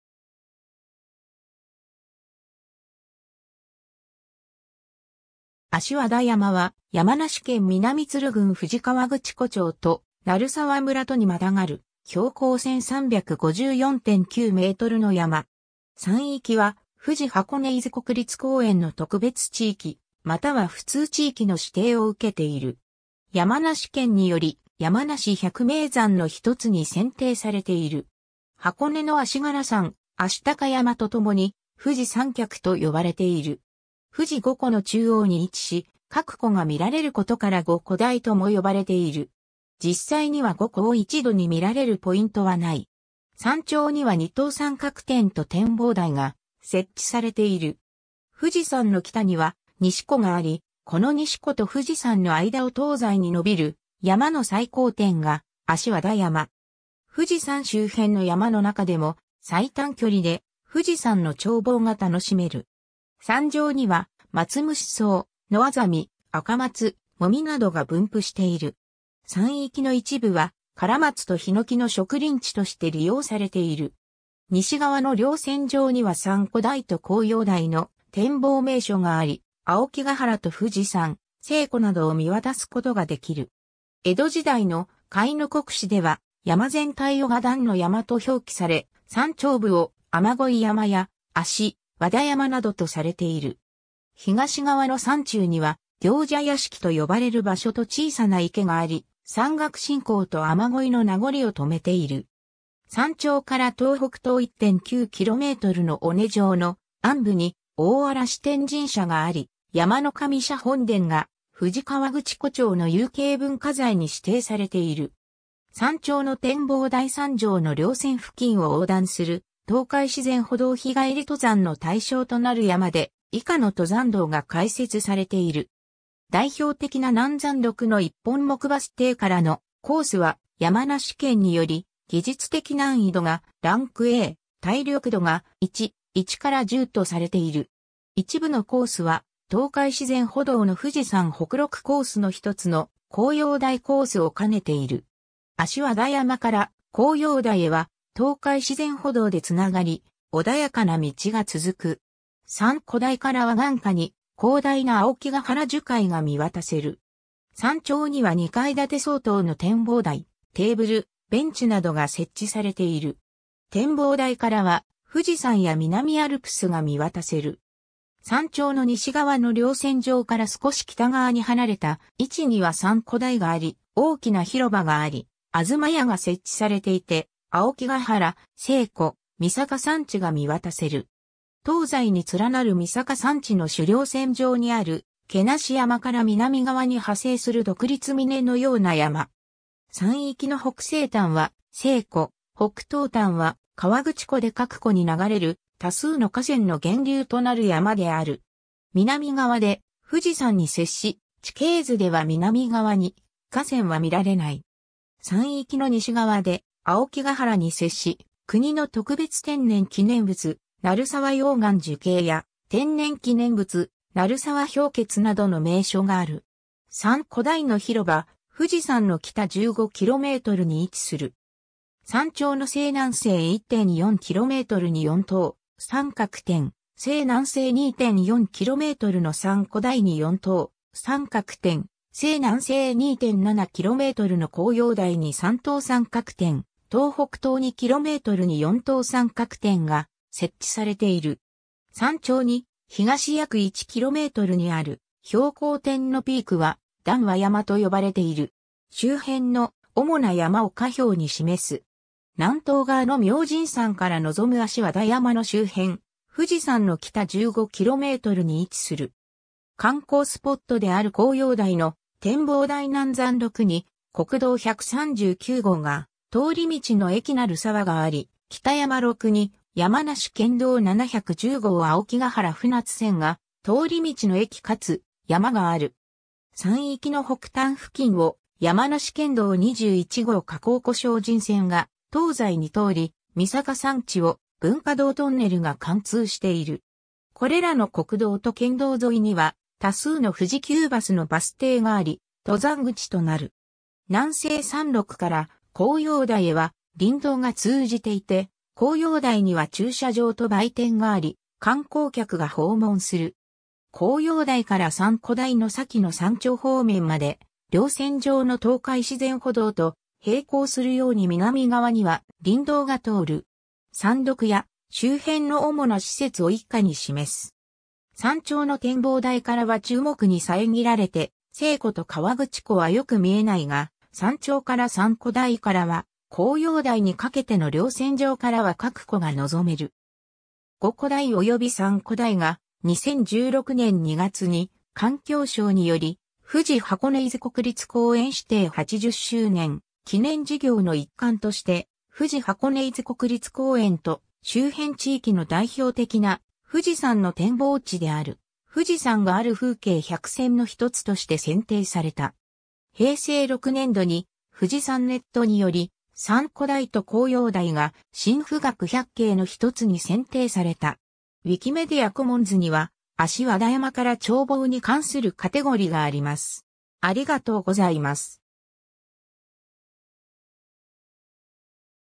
足和田山
足和田山（あしわだやま）は、山梨県南都留郡富士河口湖町と鳴沢村とにまたがる標高1,354.9 mの山。
 山域は富士箱根伊豆国立公園の特別地域または普通地域の指定を受けている。
山梨県により山梨百名山の一つに選定されている。
箱根の足柄山、愛鷹山と共に「富士三脚（富士三足）」と呼ばれている。
富士五湖の中央に位置し、各湖が見られることから五湖台とも呼ばれている。
実際には五湖を一度に見られるポイントはない。
山頂には二等三角点（点名が「檀ノ山」、標高1,354.91 m）と展望台が設置されている。
富士山の北には西湖があり、この西湖と富士山の間を東西に伸びる山の最高点が足和田山。
富士山周辺の山の中でも最短距離で富士山の眺望が楽しめる。
山上にはマツムシソウ、ノアザミ、アカマツ、モミなどが分布している。
山域の一部は、カラマツとヒノキの植林地として利用されている。
西側の稜線上には三湖台と紅葉台の展望名所があり、青木ヶ原と富士山、西湖などを見渡すことができる。
江戸時代の『甲斐国志』では、山全体をが壇ノ山（ダンノ山）と表記され、山頂部を雨乞山や足和田山などとされている。
東側の山中には行者屋敷と呼ばれる場所と小さな池があり、山岳信仰と雨乞いの名残を留めている。
山頂から東北東1.9 kmの尾根上の鞍部に大嵐天神社があり、「山神社本殿」が富士河口湖町の有形文化財に指定されている。
 山頂の展望台 山上の稜線付近を横断する東海自然歩道 日帰り登山の対象となる山で、以下の登山道（ハイキングコース）が開設されている。
代表的な南山麓の一本木バス停からのコース（無雪期・天候良好時）は、山梨県により技術的難易度が「ランクA/(A-E)」（低い-中程度）、体力度が「1/1-10」（低程度、日帰りが可能）とされている。
一部のコースは、東海自然歩道の富士山北麓コースの一つの紅葉台コース（紅葉台 - 三湖台 - 足和田山 - 大和田）を兼ねている。
足和田山から紅葉台へは東海自然歩道でつながり、穏やかな道が続く。
三湖台からは眼下に広大な青木ヶ原樹海が見渡せる。
山頂には2階建て相当の展望台、テーブル、ベンチなどが設置されている。
展望台からは富士山や南アルプスが見渡せる。
山頂の西側の稜線上から少し北側に離れた位置には三湖台があり、大きな広場があり東屋が設置されていて青木ヶ原、西湖、御坂山地が見渡せる。
 東西に連なる御坂山地の主稜線上にある毛無山（標高1,500 m）から南側に派生する独立峰のような山。
山域の北西端は西湖、北東端は河口湖で各湖に流れる多数の河川の源流となる山である。
南側で富士山に接し、地形図では南側に河川は見られない。
山域の西側で青木ヶ原に接し、国の特別天然記念物『鳴沢熔岩樹型』や天然記念物『鳴沢氷穴』などの名所がある。
 三湖台（標高1,202 m）の広場 富士山の北15 kmに位置する。
山頂の西南西1.4 kmに四等三角点（点名が「鳴沢」、標高1237.49 m）、西南西2.4 kmの三湖台に四等三角点（点名が「紅葉台」、標高1202.48 m）、西南西2.7 kmの紅葉台に三等三角点（点名が「物見所」、標高1164.63 m）、東北東2 kmに四等三角点（点名が「一之瀬」、標高946.54 m）が設置されている。
山頂に東約1 kmにある標高点（1,233 m）のピークは段和山と呼ばれている。
周辺の主な山を下表に示す。
 南東側の明神山から望む足和田山の周辺、富士山の北15 kmに位置する。
 観光スポットである紅葉台の展望台 南山麓に国道139号が通り道の駅なるさわがあり、北山麓に山梨県道710号青木ヶ原船津線が通り道の駅かつやまがある。
山域の北端付近を山梨県道21号河口湖精進線が東西に通り御坂山地を文化洞トンネルが貫通している。
これらの国道と県道沿いには多数の富士急バスのバス停があり、登山口となる。
南西山麓から紅葉台へは林道が通じていて、紅葉台には駐車場と売店（屋上に展望台）があり、観光客が訪問する。
紅葉台から三湖台の先の山頂方面まで稜線上の東海自然歩道と並行するように南側には林道が通る。
 山麓や周辺の主な施設を以下に示す。
 山頂の展望台からは樹木に遮られて、西湖と河口湖はよく見えないが、山頂から三湖台からは紅葉台にかけての稜線上からは各湖が望める。
五湖台（足和田山）および三湖台が、2016年（平成28年）2月に環境省により、富士箱根...